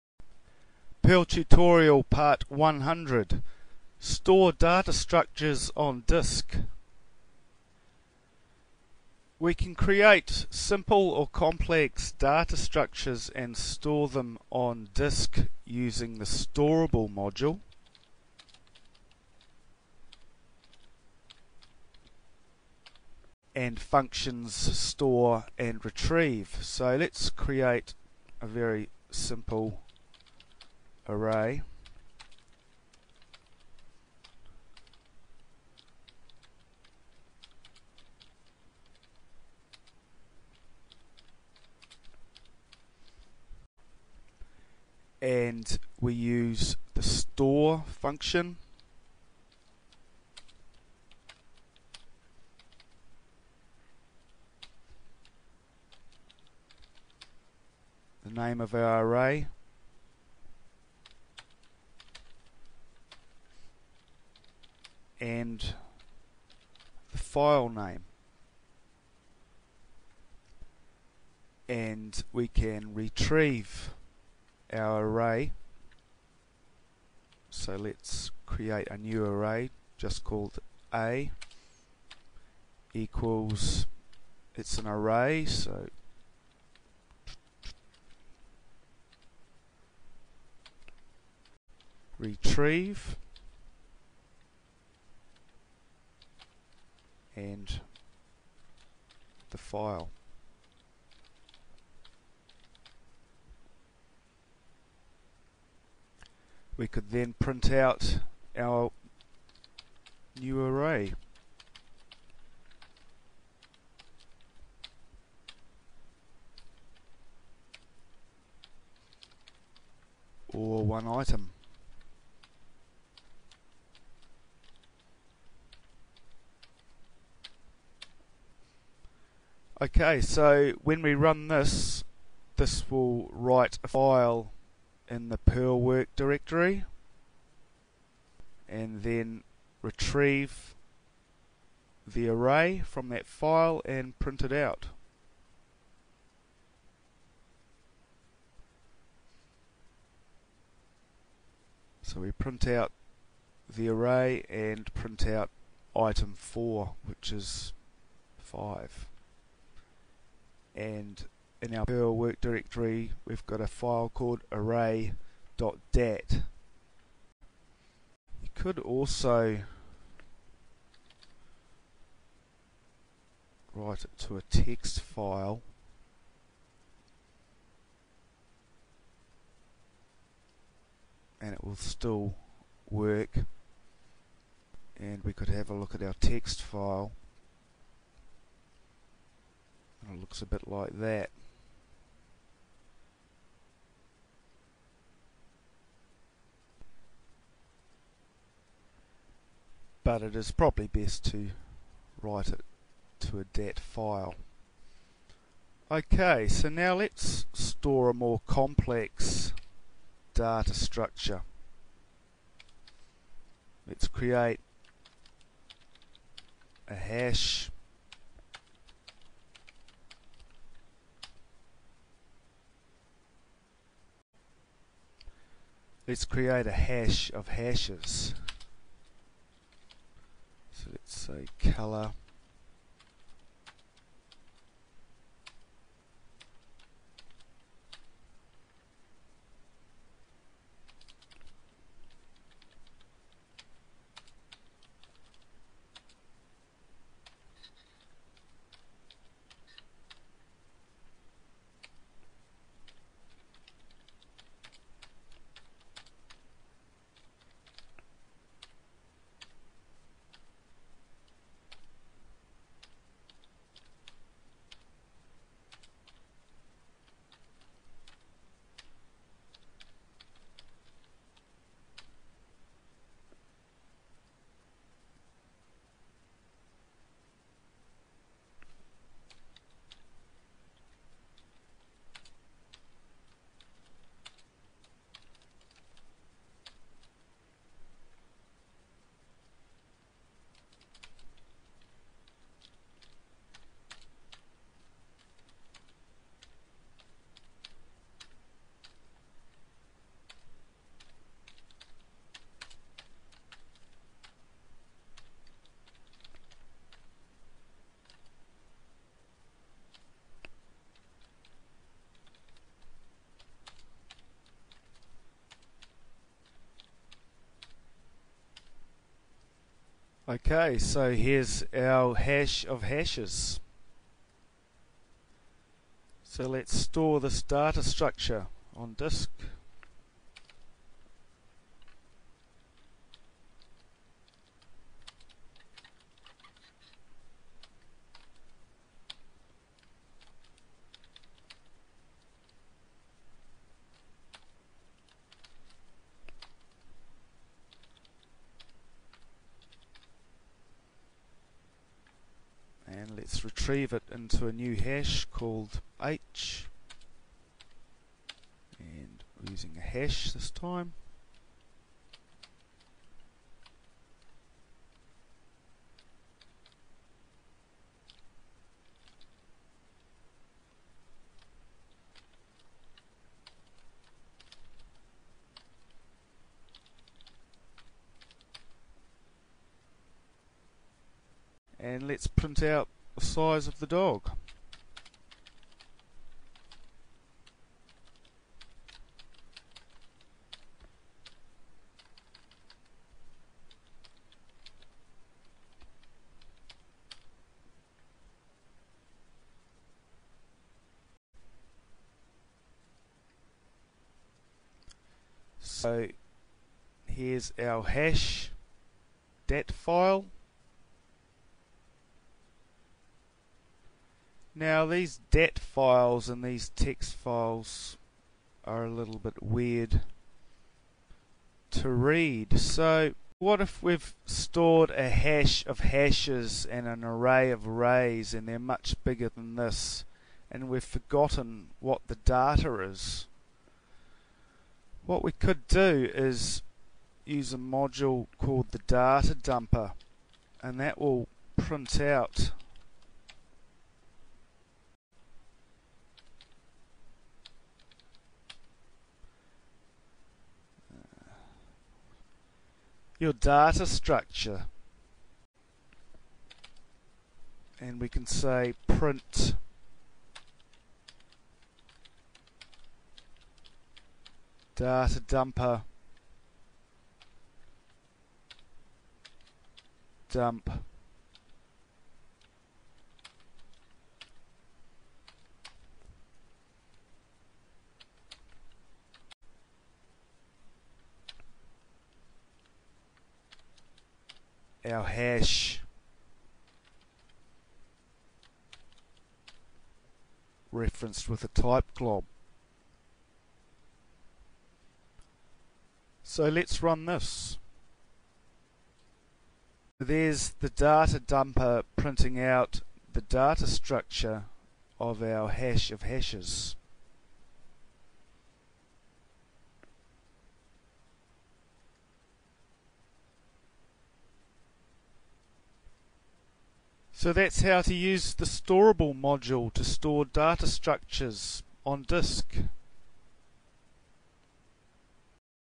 Perl Tutorial 100 - Store Data Structures on Disk
Part 100 of the Perl Tutorial shows how to store data structures on disk with the storable module and how to retrieve them later. South America Photo Map (Free Android App)